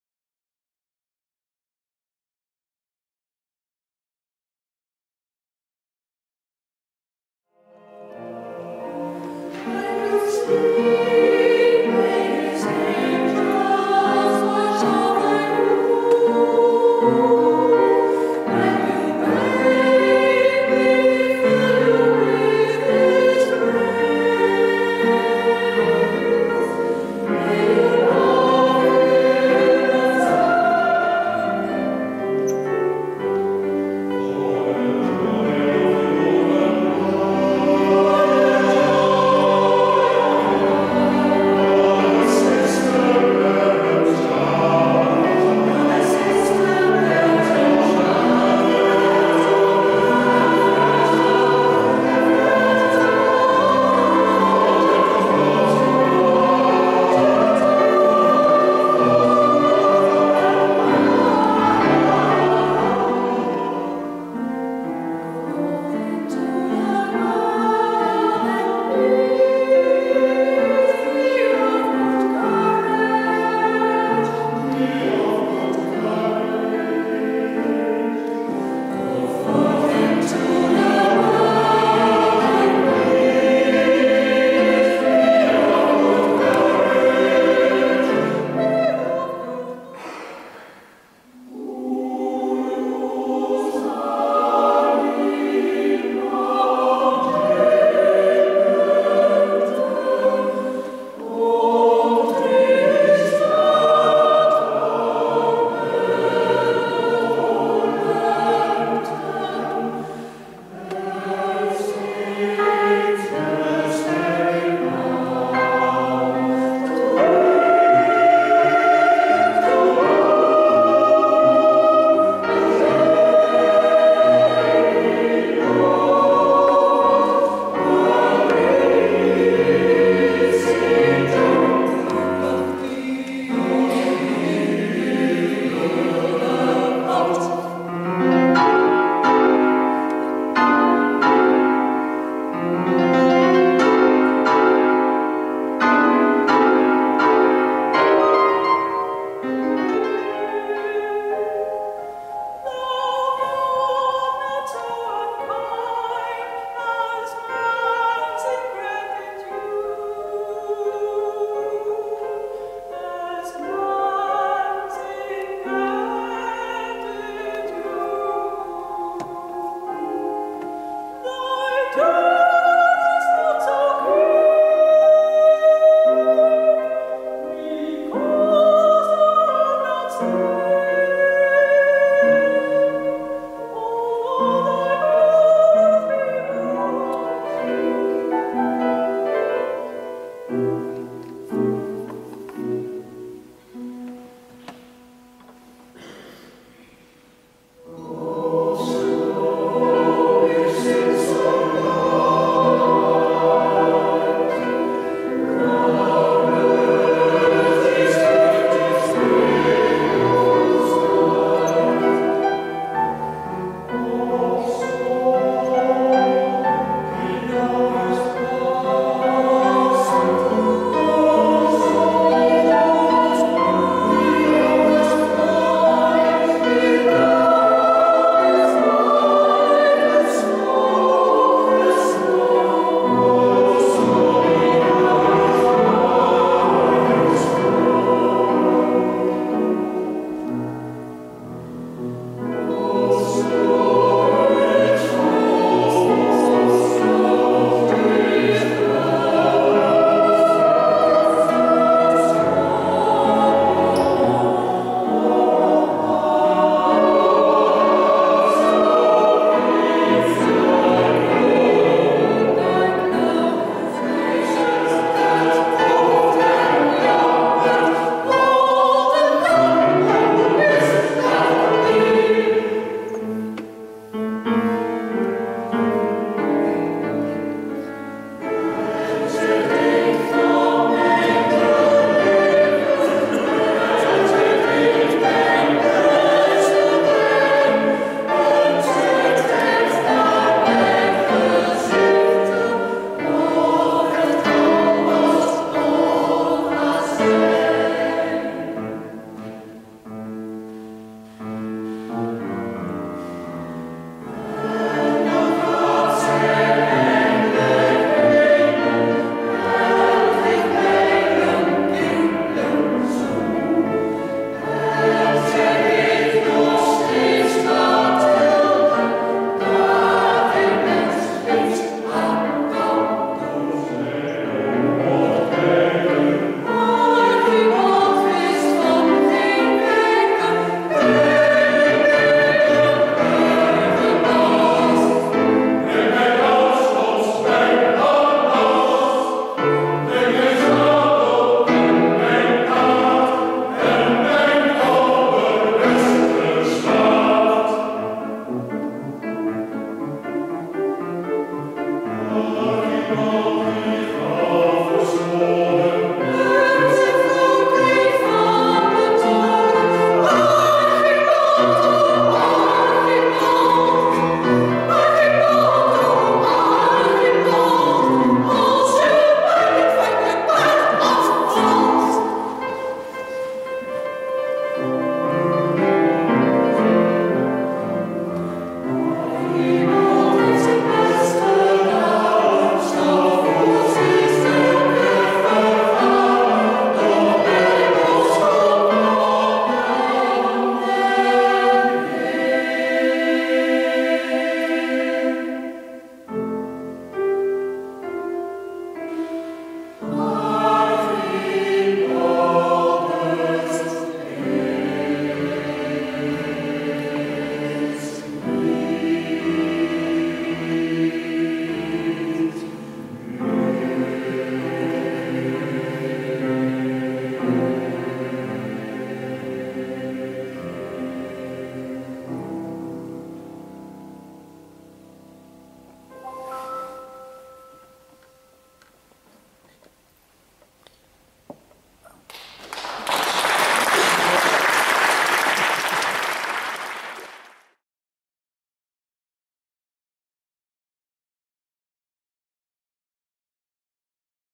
Fibonacci Xaveriuskerk 12 maart 2016
Kamerkoor Fibonacci o.l.v. Marijke van Leerzem geeft een kort optreden in de FRanciscus Xaveriuskerk in Amersfoort op 12 maart 2016. Solist: Tine Lempens